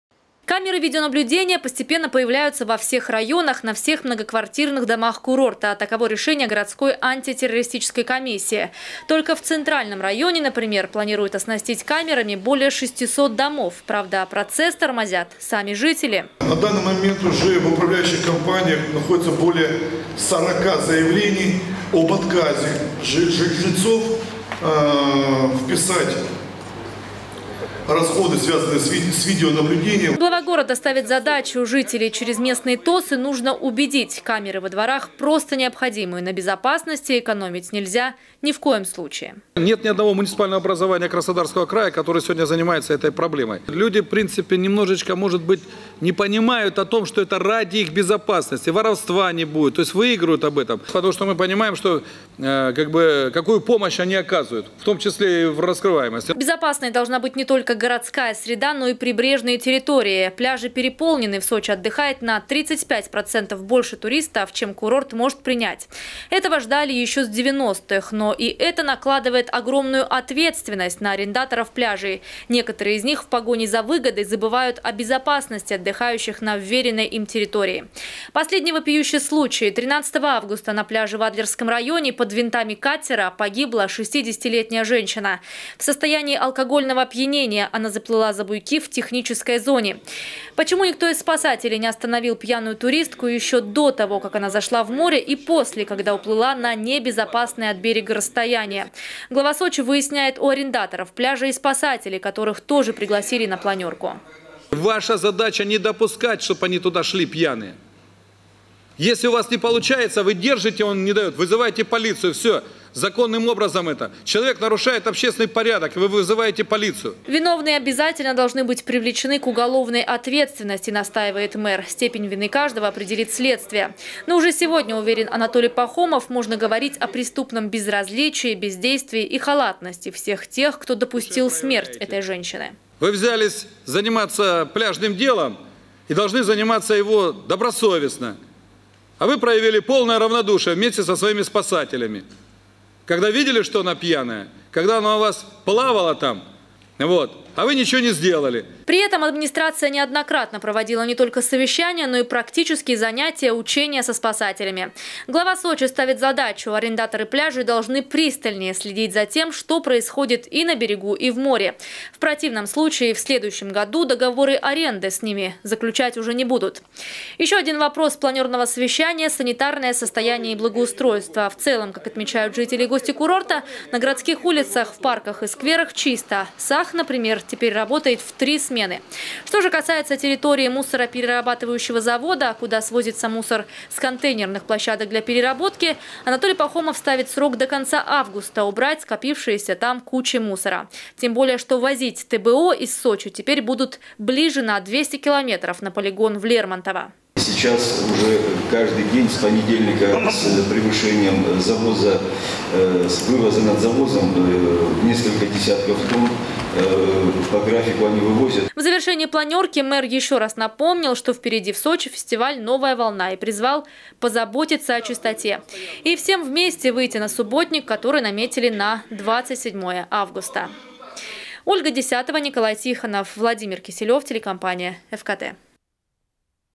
Вопрос безопасности стал главным на планерке у мэра. Новости Эфкате Сочи
Затронули тему безопасности на пляжах и во дворах домов, где продолжают устанавливать камеры видеонаблюдения.

Они постепенно появляются во всех районах, на всех многоквартирных домах курорта — таково решении городской Антитеррористической комиссии. Только в Центральном районе, например, планируют оснастить камерами более 600 домов. Правда, процесс тормозят сами жители.

Сочи. Новости.
Телеканал Эфкате.